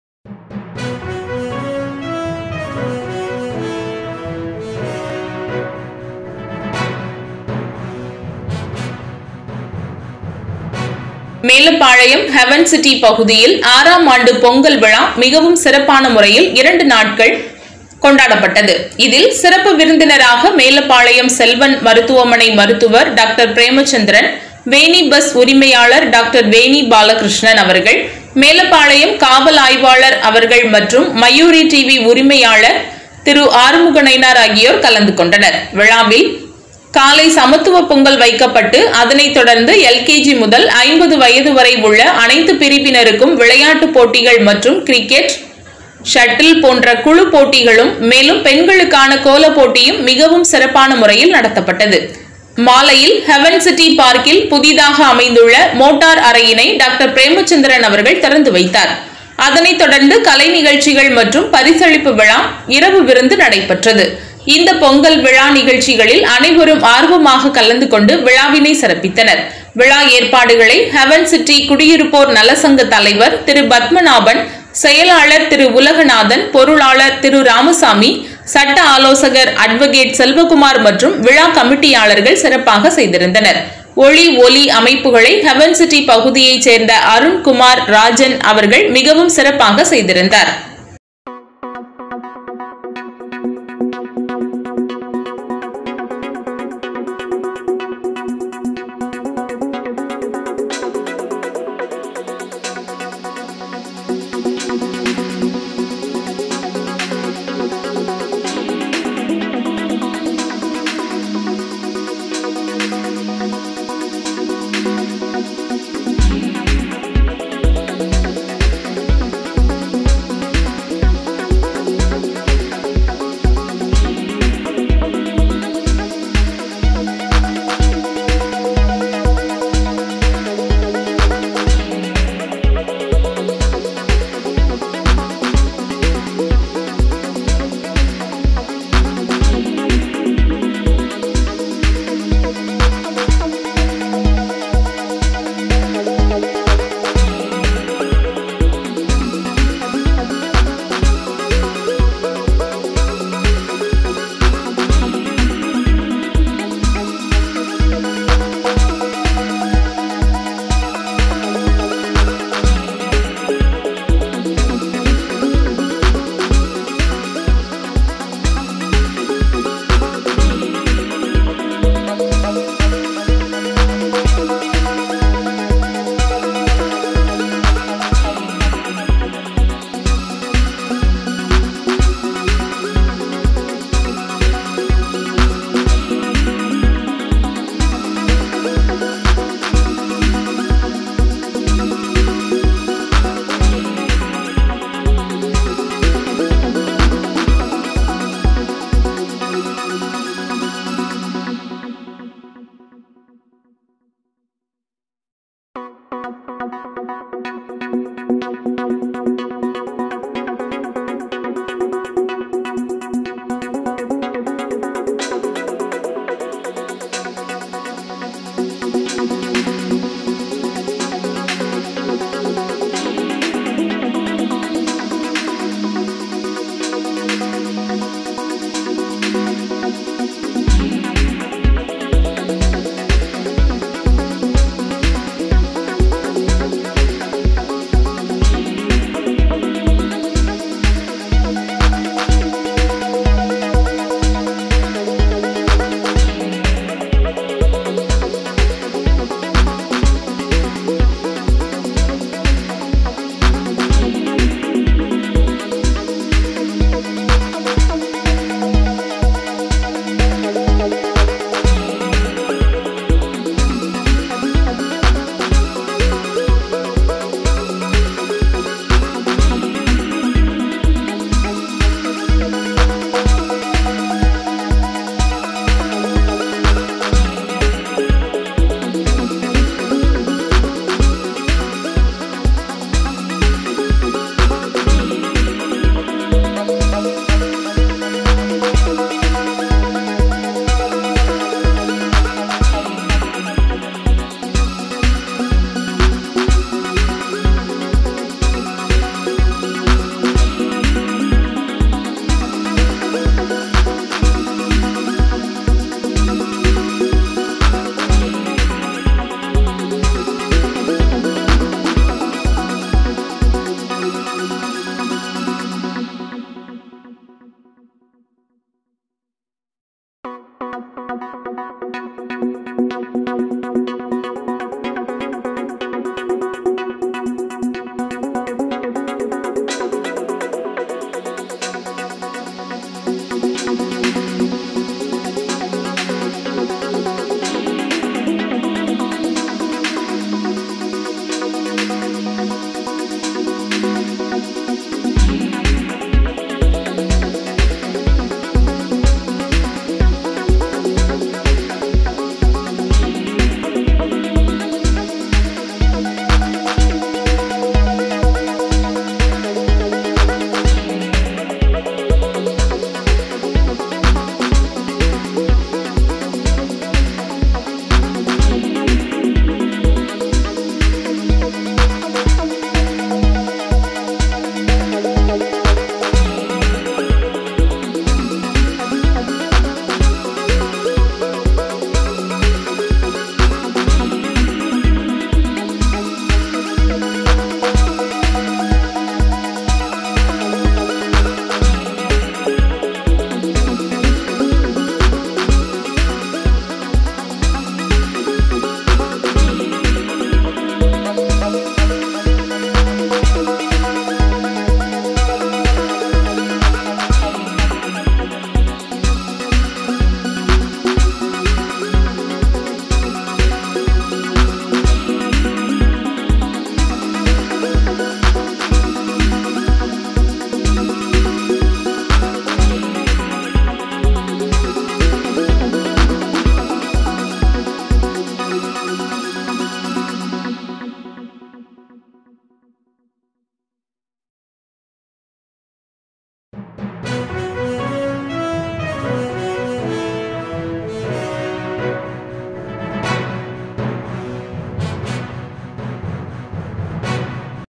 ஹெவன் சிட்டி பகுதியில் 6ம் ஆண்டு பொங்கல் விழா
மேலப்பாளையம்.ஹெவன் சிட்டி பகுதியில் 6ம் ஆண்டு பொங்கல் விழா மிகவும்  சிறப்பான முறையில் 2 நாட்கள் ஜனவரி 22&23 தேதிகளில் கொண்டாடப் பட்டது.இதில் சிறப்பு விருந்தினராக மேலப்பாளையம் செல்வன் மருத்துவ மனை மருத்துவர் Dr.பிரேமசந்திரன் ,வேணி பஸ் உரிமையாளர் Dr.வேணி பாலகிருஷ்ணன் அவர்கள்,மேலப்பாளையம் காவல் ஆய்வாளர் அவர்கள் மற்றும் மயூரி டிவி உரிமையாளர் திரு.ஆறுமுகநயினார் அவர்கள் கலந்துகொண்டனர்.விழாவில் காலை சமத்துவ பொங்கல் வைக்கப்பட்டு அதனை தொடர்ந்து LKG முதல் 50வயது வரை உள்ள அனைத்து பிரிவினருக்கும் விளையாட்டுப் போட்டிகள் மற்றும் கிரிக்கெட் ,ஷட்டில் போன்ற குழு போட்டிகளும் மேலும் பெண்களுக்கான கோலப்போட்டியும் மிகவும் சிறப்பான முறையில்  நடத்தப் பட்டது..23 மாலை 6 மணியளவில் ஹெவன் சிட்டி பார்கில் புதிதாக அமைந்துள்ள மோட்டார் அறையினை Dr.பிரேமசந்திரன் அவர்கள் திறந்து வைத்தார்கள. அதனை தொடர்ந்து கலை  நிகழ்ச்சிகள் மற்றும் பரிசளிப்பு விழா , இரவு விருந்து நடை பெற்றது. .இந்த பொங்கல் விழா நிகழ்ச்சிகளில் அனைவரும் ஆர்வமாக கலந்து கொண்டு விழாவினை சிறப்பித்தனர்.

விழா ஏற்பாடுகளை ஹெவன்சிட்டி குடியிருப்போர் நல சங்க தலைவர் திரு.பத்மநாபன் செயலாளர் திரு உலகநாதன் பொருளாளர் திரு ராமசாமி ,சட்ட ஆலோசகர் அடவகேட் செல்வகுமார் மற்றும் விழா கமிட்டி யாளர்கள் சிறப்பாக செயதிருந்தினர்...

ஒளி,ஓலி அமைப்புகளை ஹெவன் சிட்டி பகுதியை சேர்ந்த அருண்குமார் ராஜன் அவர்கள் மிகவும் சிறப்பாக செய்திருந்தார்கள..